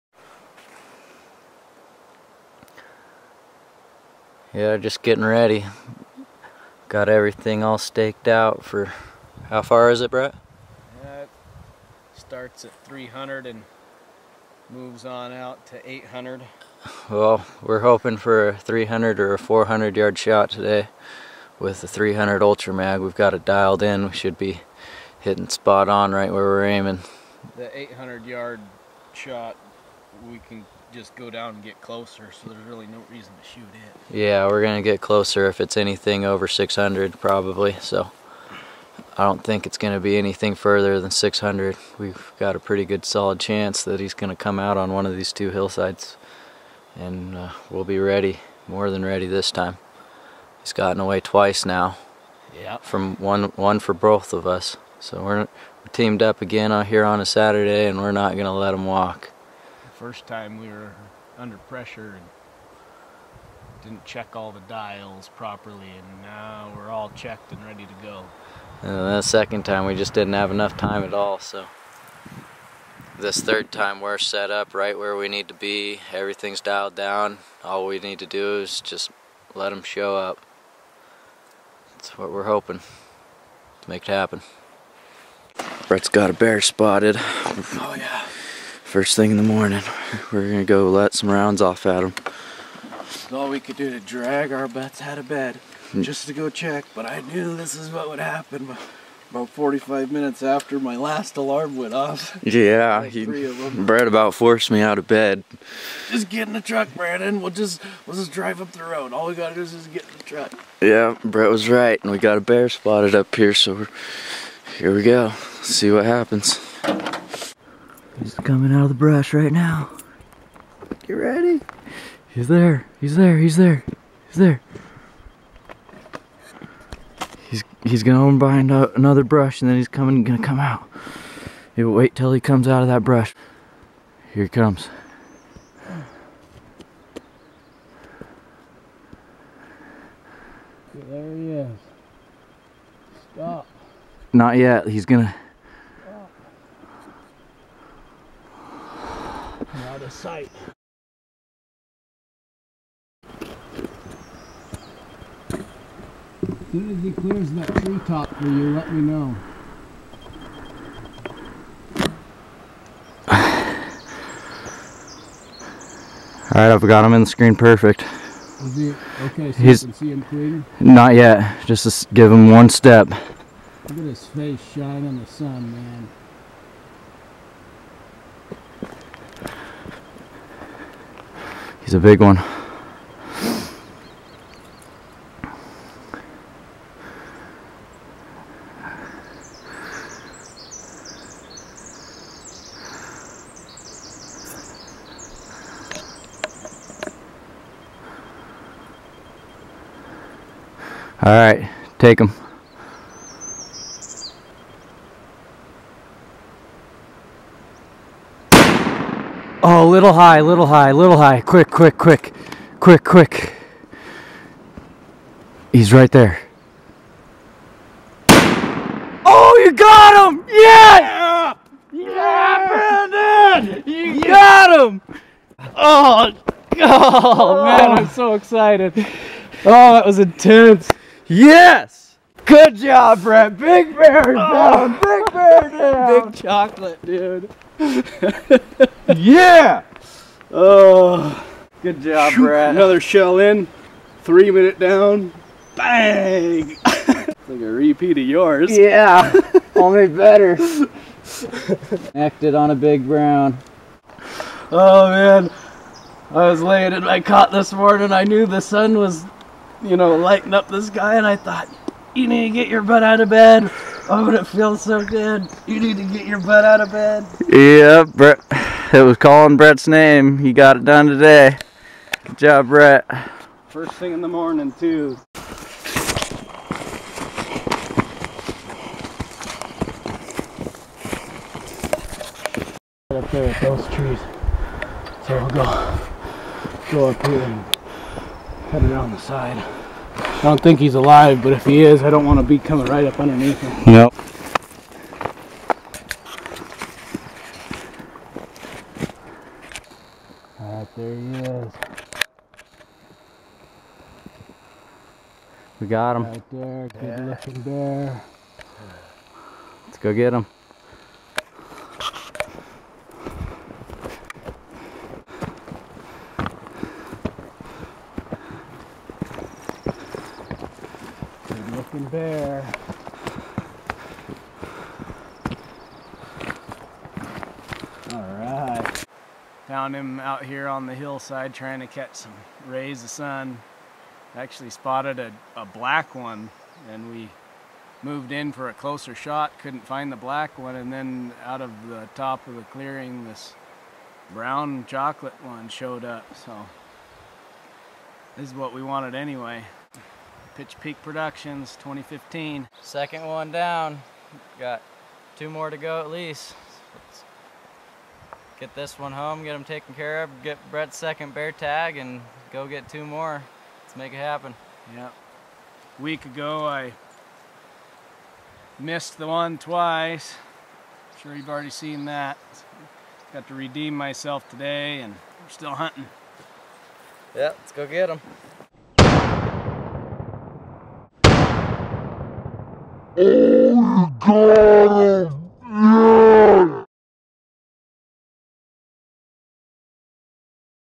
2015 North Idaho Spring Bear Movie Part 2, Brandon Pitcher, Pitch Peak Productions ep. 11
"Warning" contains graphic footage.  Brett Pitcher and Brandon Pitcher shoot a big Chocolate colored Black Bear in northern Idaho spot and stalk. May 3rd, 2015.  Pitch Peak Productions.